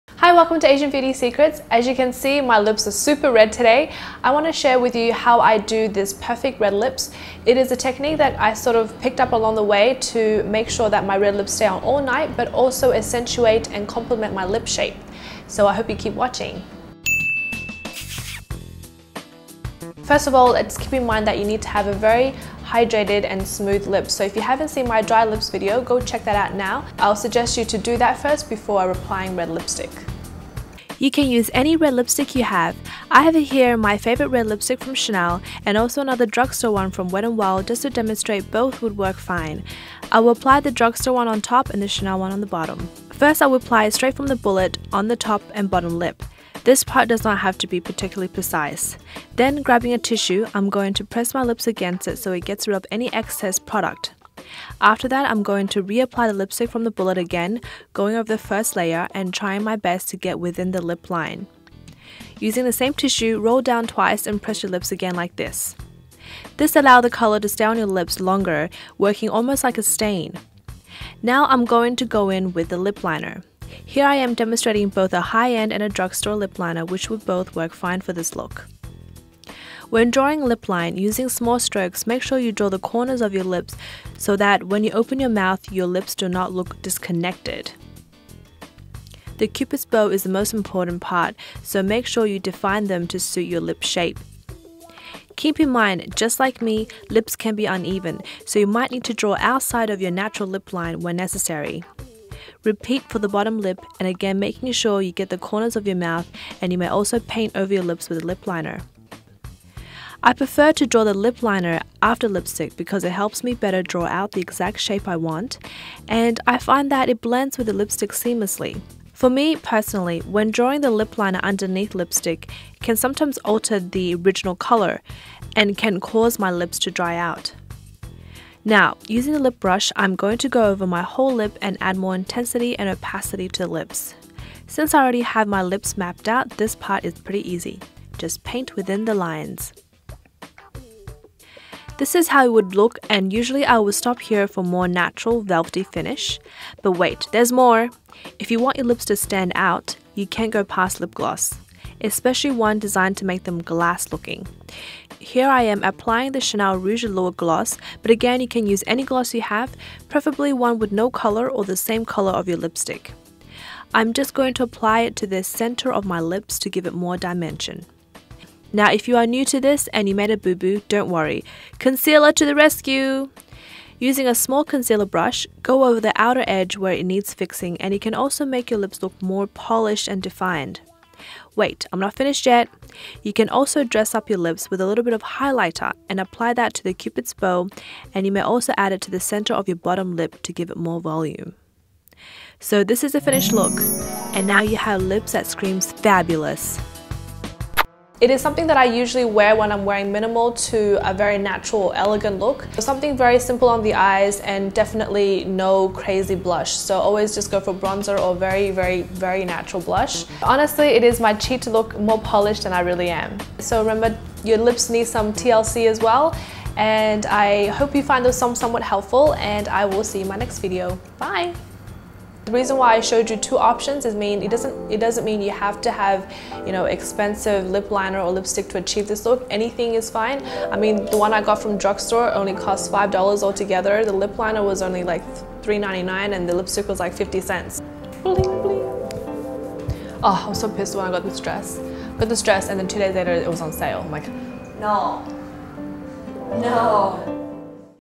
How to Do Perfect Red Lips
Muuuuah~~~
So the red lip look is gorgeous and bold and anyone can pull it off effortlessly. Today, I am showing you my tutorial on how to get the perfect long lasting red lips using only a few tools.
Let me know if this works for you, and get ready to rock the red lip look!! :D

List of Products:
Chanel Rouge Allure Velvet #38 La Fascinante
Wet n Wild in HOT RED #519 A
Chanel Le Crayon Levres #14 Carmin
Milani Color Statement Lipliner #02 True Red
Chanel Rouge Allure Gloss #60 Excess
Chanel White Essential Concealer #10
Highlighter - Lumiere Sculptee De Chanel

Some secrets are better shared than kept